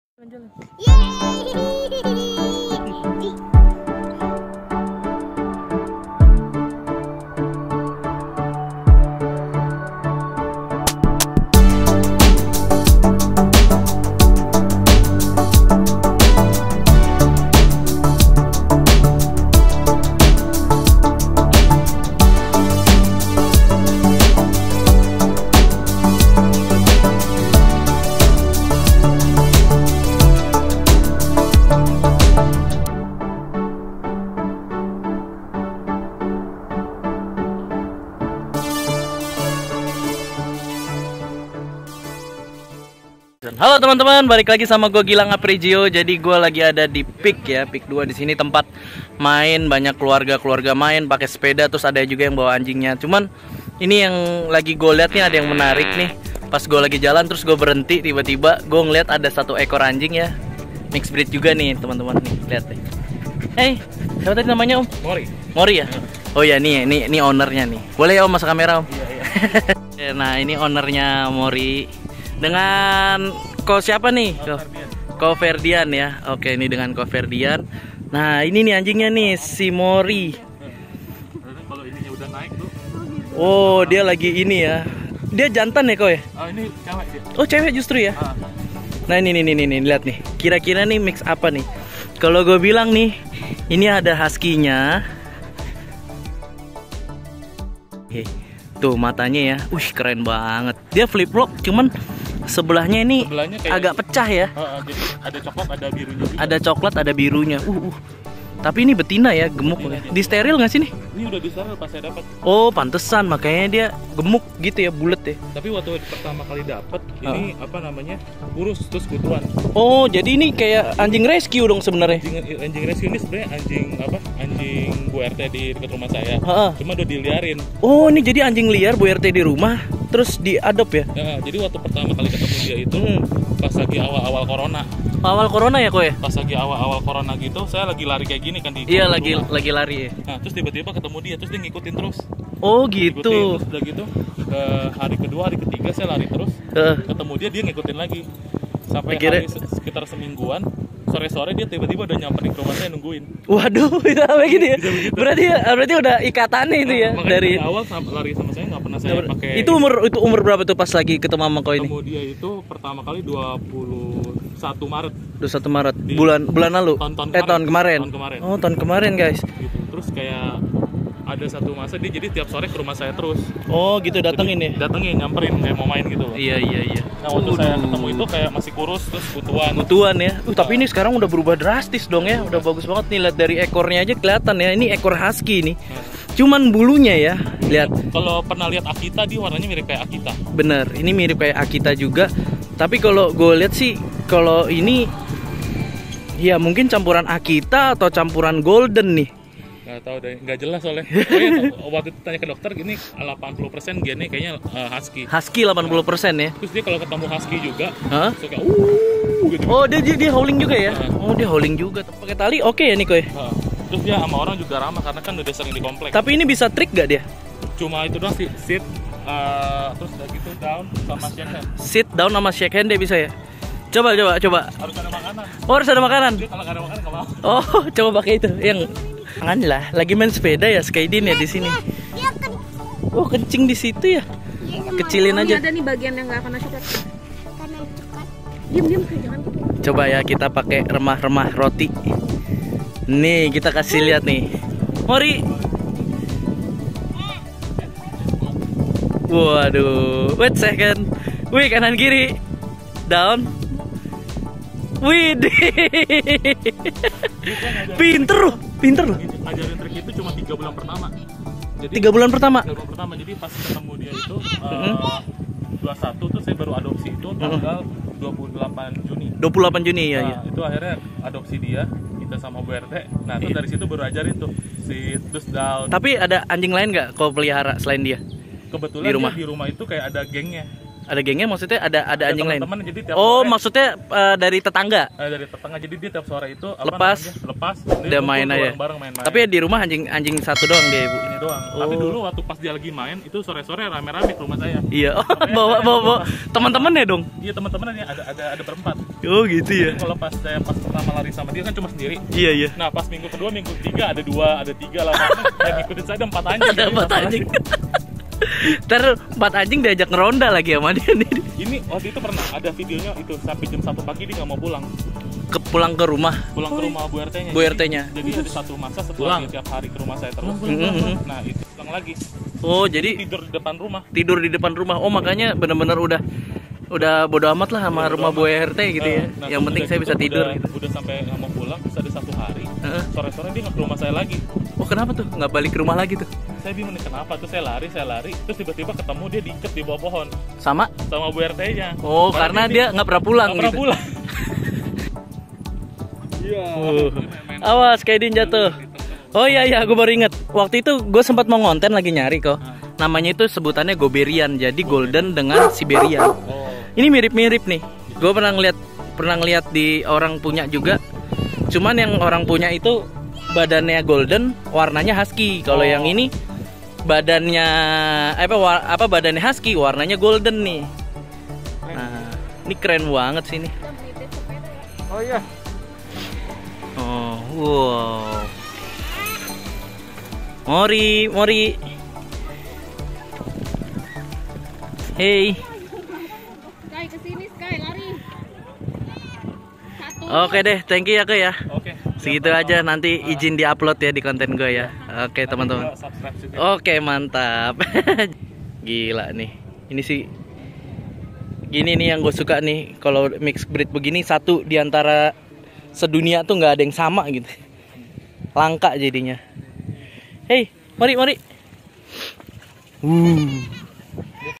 GOBERIAN ‼️ DI TELANTARIN 😱 INI MIXBREED MAHAL NYESEL KALAU TAU
GOBERIAN ‼️ DI TELANTARIN BU RT PADAHAL INI MIXBREED MAHAL 😱

Halo Brother ku... Kali ini gw ketemu dengan salah satu anjing unik banget 😱 gak sengaja bener gw lagi jalan2 sm keluarga terus ketemu dia... Namanya Morry dia ini baru aja di adopsi sm ownernya dari Bu RT di rumah nya 😂 kasian lohh tadinya kurus dan kutuan trus sampai akhirnya di rawat oleh bro Ferdian sampai jadi se cakep ini 🤗 Oh ya ini Mix breed antara Golden X Husky ya atau yg di sebut di Luar negri GOBERIAN 🔥🔥🔥

NONTON DEHH BIAR GAK PENASARAN ⁉️